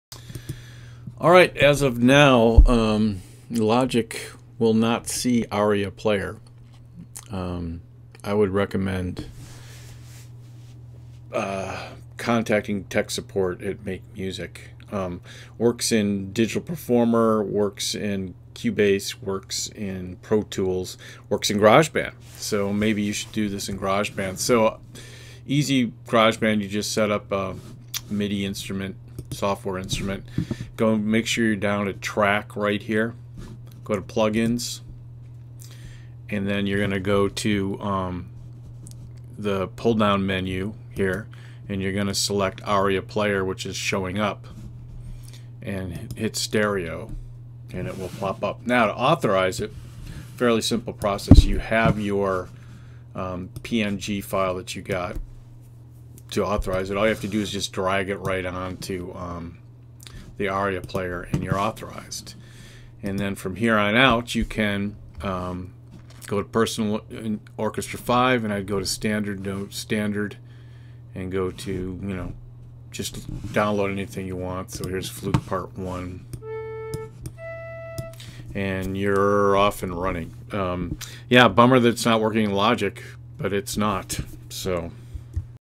Aria Player and Logic
Aria Player not working on Logic with Sierra AND High Sierra. One would think that the MakeMusic folks would have fixed this by now. Especially on Sierra!